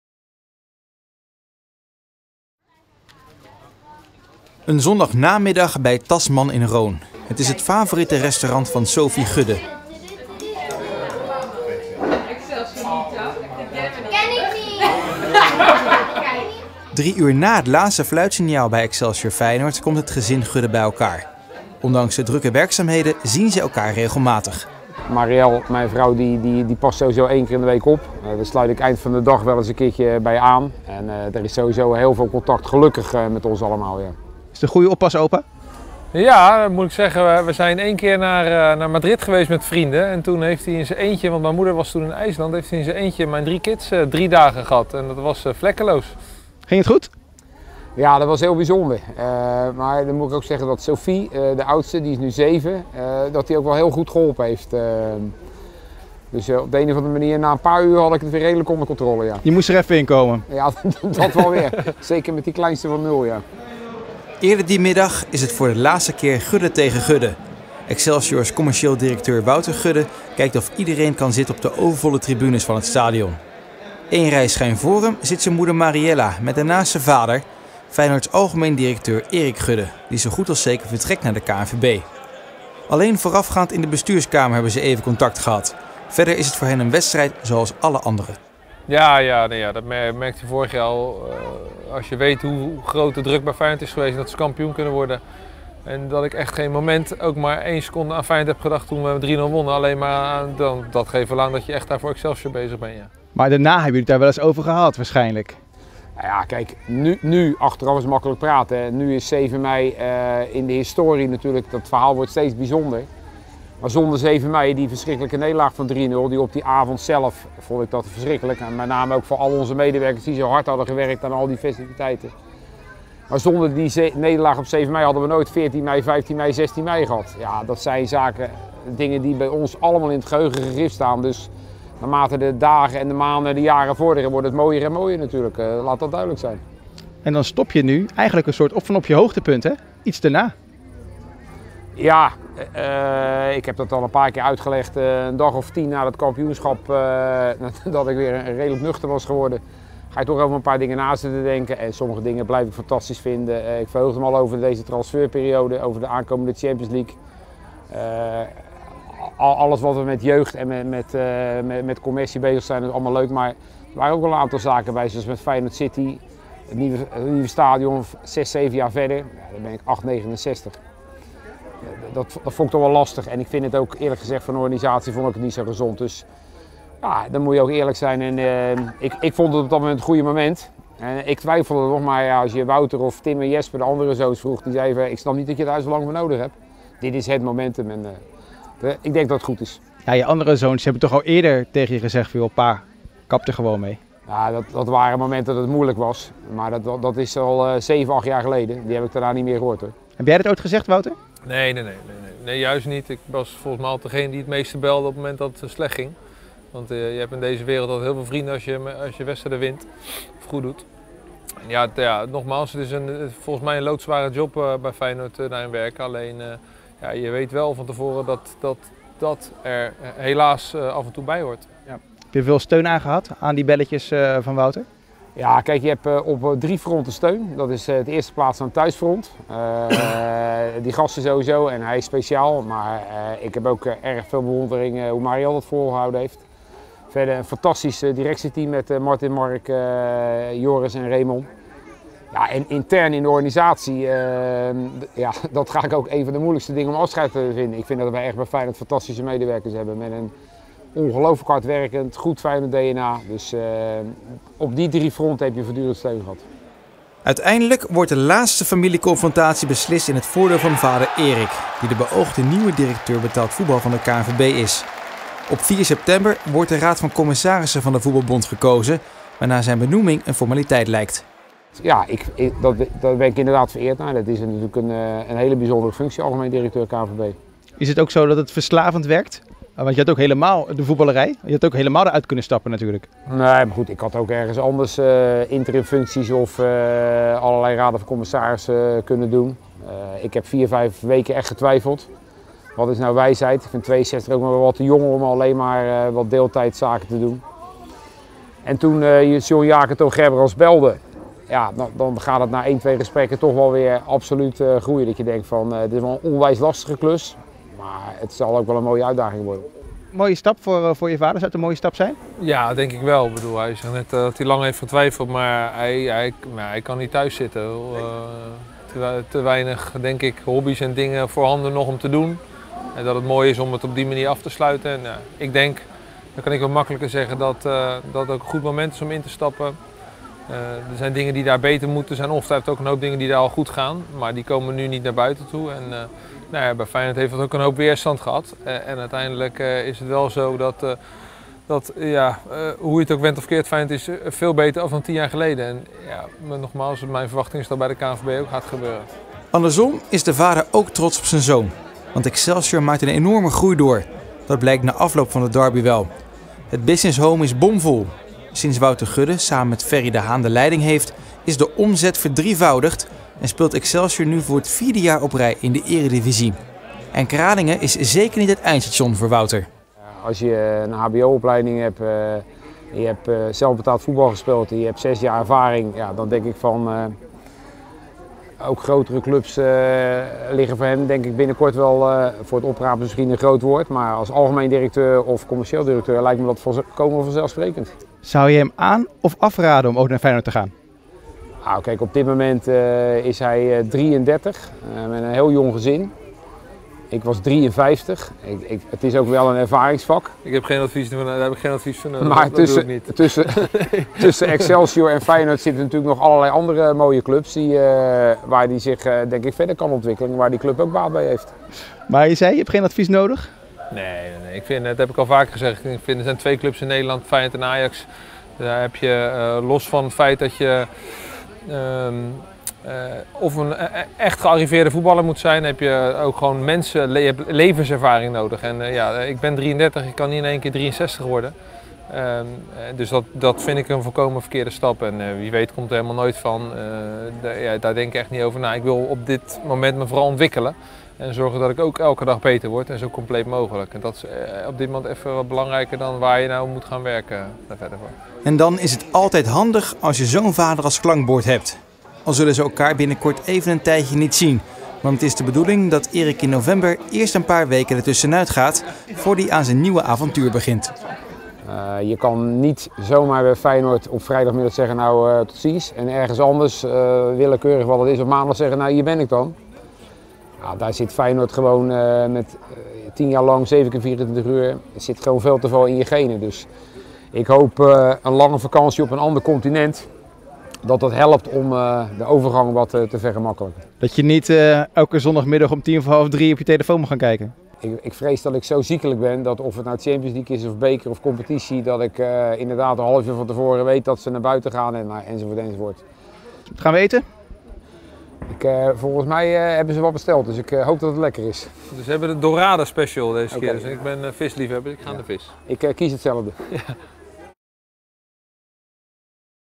Gudde & Gudde: openhartig na laatste derby
Excelsior-Feyenoord was de laatste keer dat Eric Gudde en Wouter Gudde tegenover elkaar stonden. Vader Eric verruilt het algemeen directeurschap bij Feyenoord zo goed als zeker voor een baan bij de KNVB. Zijn zoon Wouter is commercieel directeur bij Excelsior en steunt hem volledig. Zoals hij dat ook gedaan heeft in zwaardere tijden.Drie uur na het laatste fluitsignaal op Kralingen heeft het gezin Gudde afgesproken bij een restaurant in Rhoon. Hoewel de directeuren een drukke baan hebben, zien zij elkaar nog geregeld.In een openhartig gesprek spreken zij over de aanstaande carrièremove van vader Eric als directeur betaald voetbal van de KNVB en de onvoorwaardelijke steun die hij kreeg van zoon Wouter en de rest van zijn gezin in een lastige periode bij Feyenoord. Maar ook spreken zij over de groei van Excelsior onder Wouter, die op 33-jarige leeftijd &apos;levenservaring mist om bij Feyenoord of Ajax aan de slag te gaan.&apos; En ook wordt er gesproken over de aanstaande vakantie van Eric en zijn rol als oppas-opa.